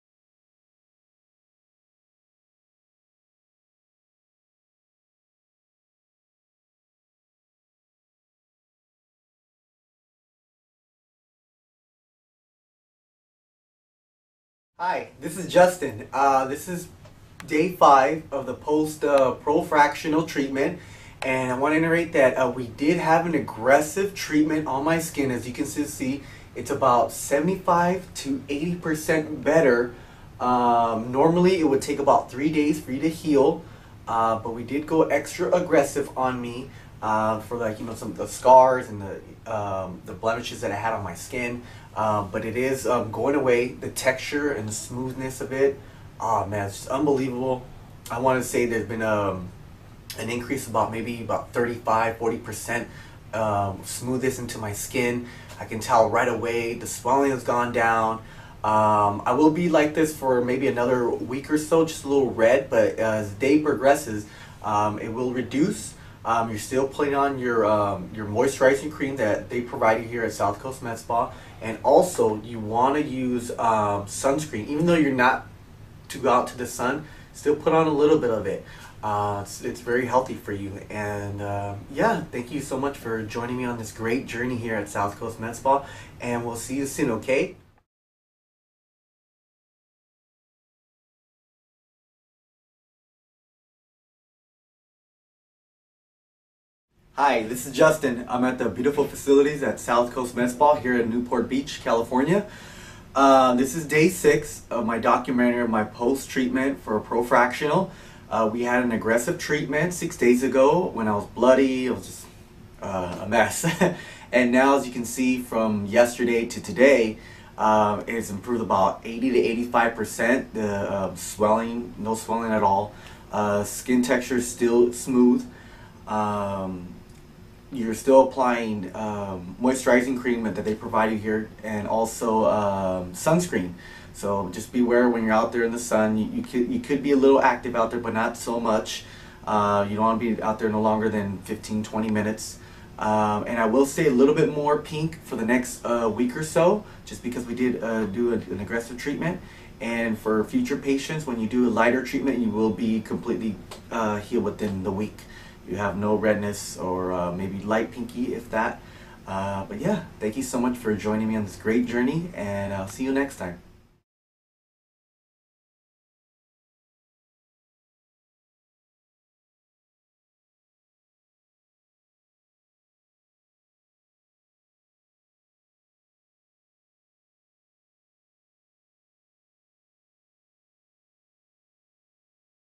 Justin documents his Post Laser Acne Scar Removal Progress at South Coast MedSpa
Justin shares his laser acne scar removal progress with interested potential patient. Justin had a very aggressive laser acne scar removal treatment using the Sciton Profractional laser at South Coast MedSpa.